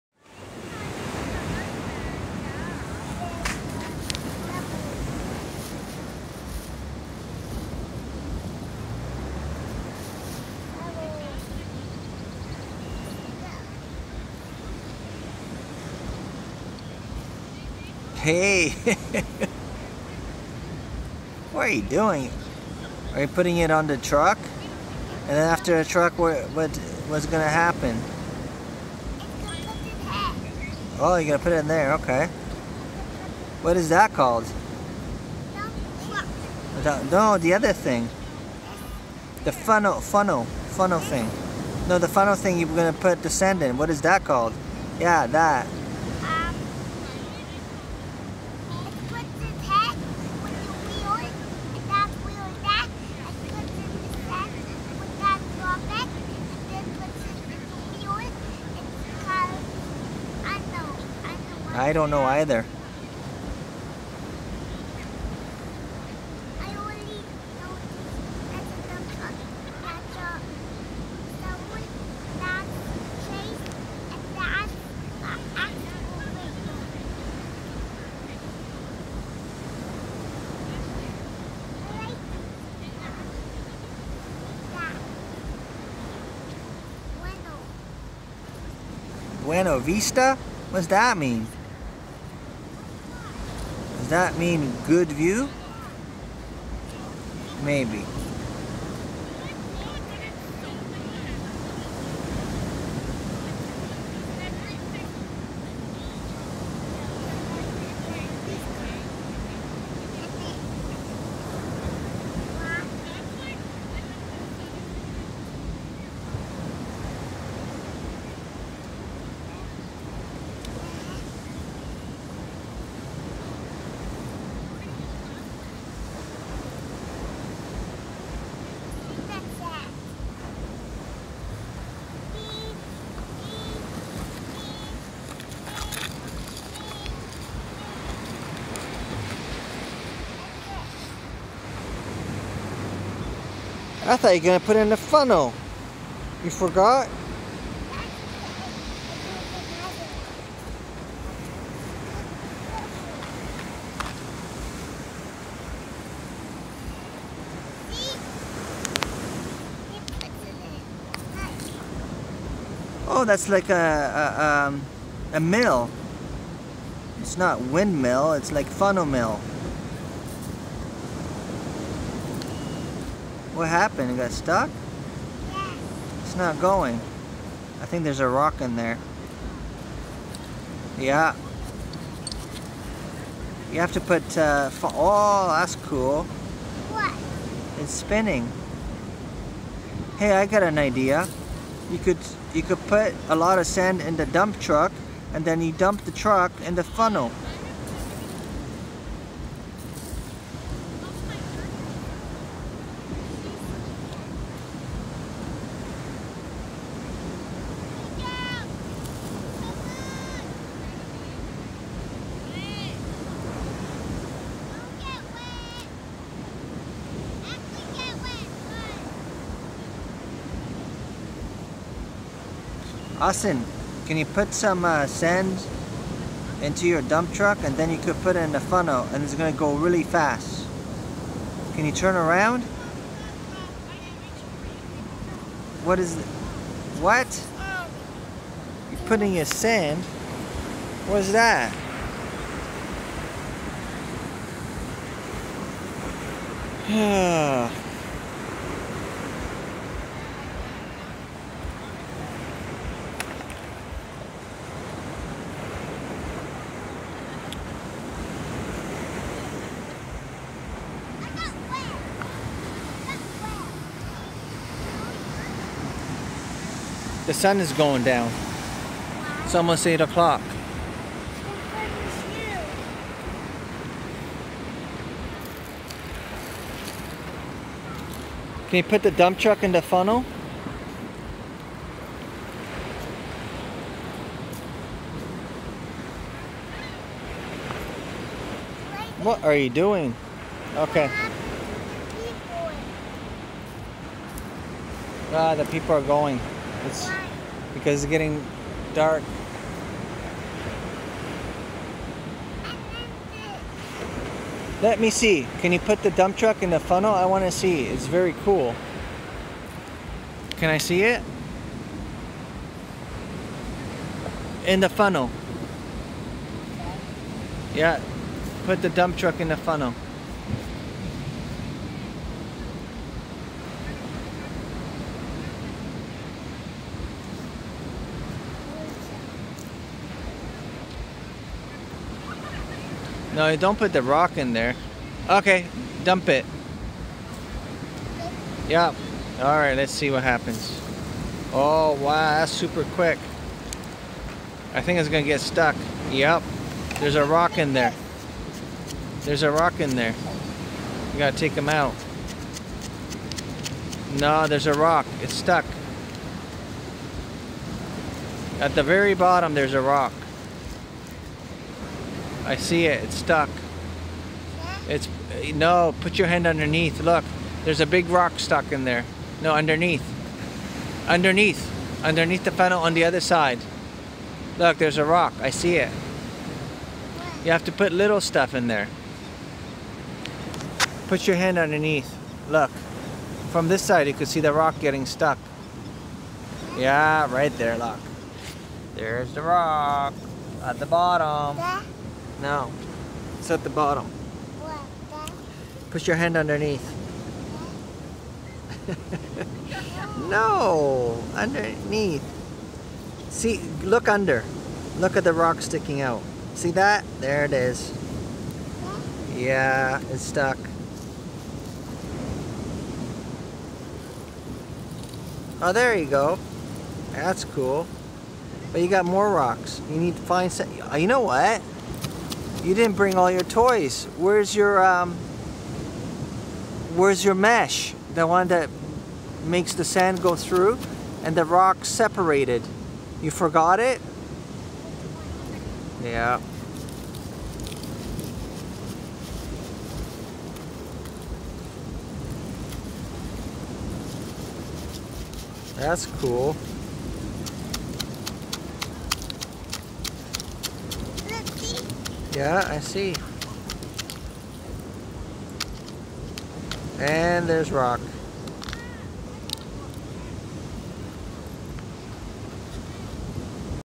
Otto playing with sand toys on the beach
Otto playing with sand toys on the beach in Hamilton Ontario